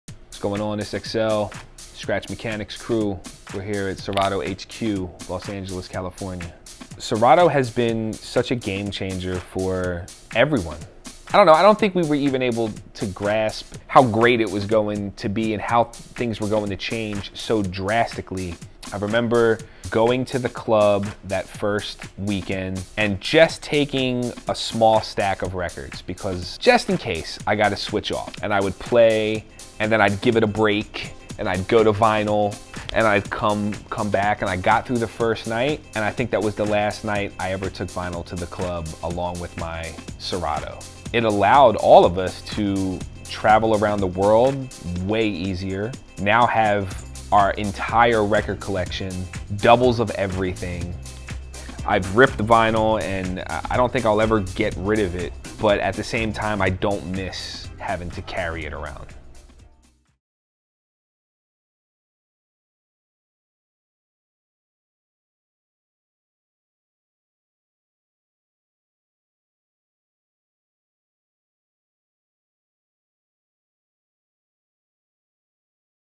1 Minute With: DJ Excel
A veteran DJ/Producer from Philadelphia, DJ Excel now lives in LA and is one of the busiest club DJs in the game. Home for a day, he stopped by our studio and discussed how Serato has affected his career as a travelling DJ over the years.